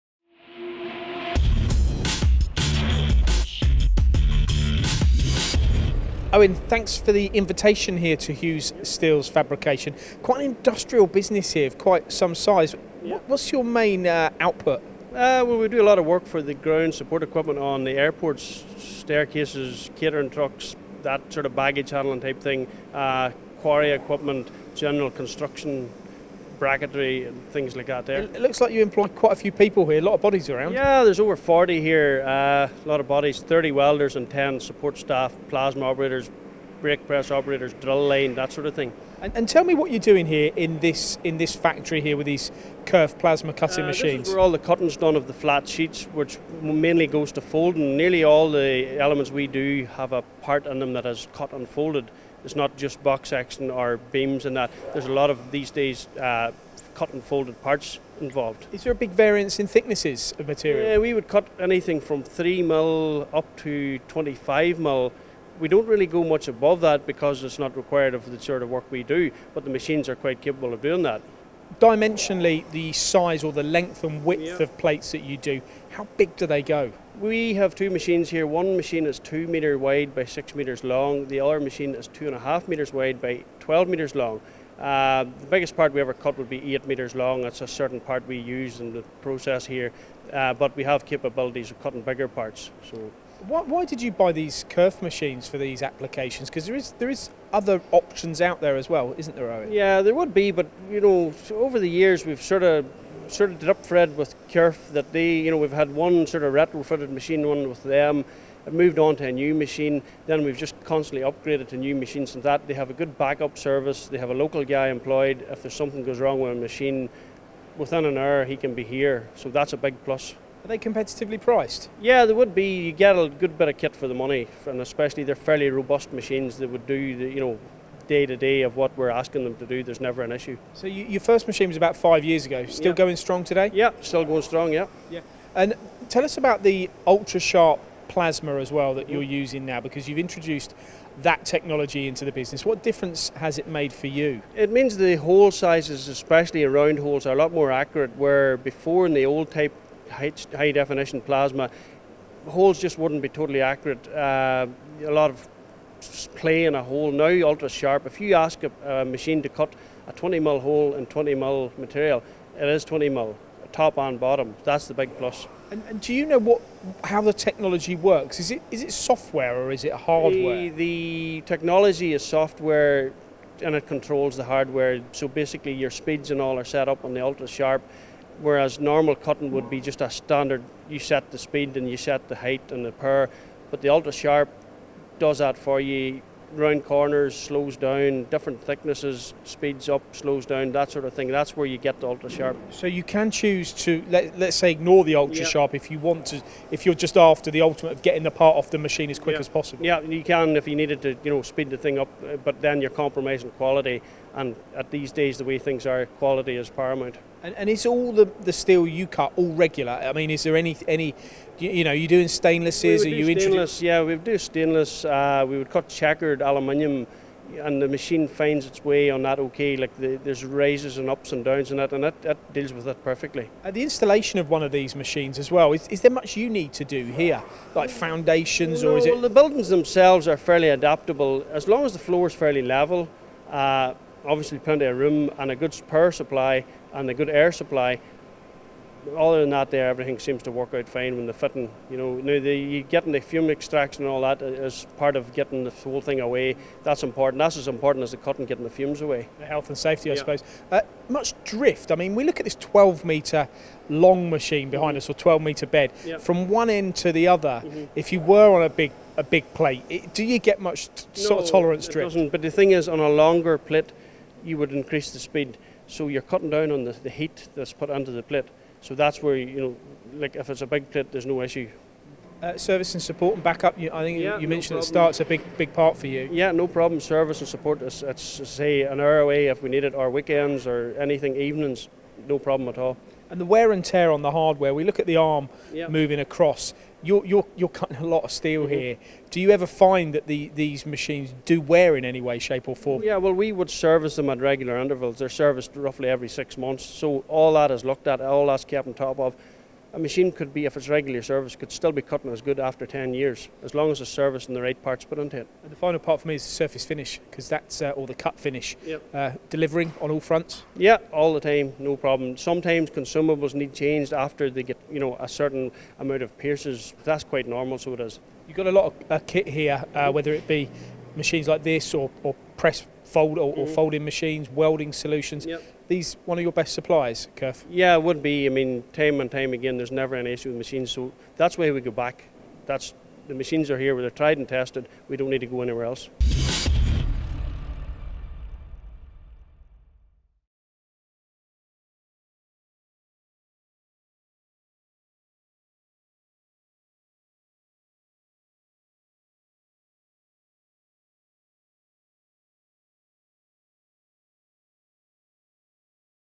Hughes steel cuts faster and with more precision thanks to Kerf
A recent visit to Northern Ireland saw MTDCNC visit Hughes Steel. A company that fabricates products for various industries including ground equipment for the aircraft sector. Specifically fabrication and sheet metal work, the company recently purchased yet another Kerf plasma cutter to support growing demands from local and overseas customers.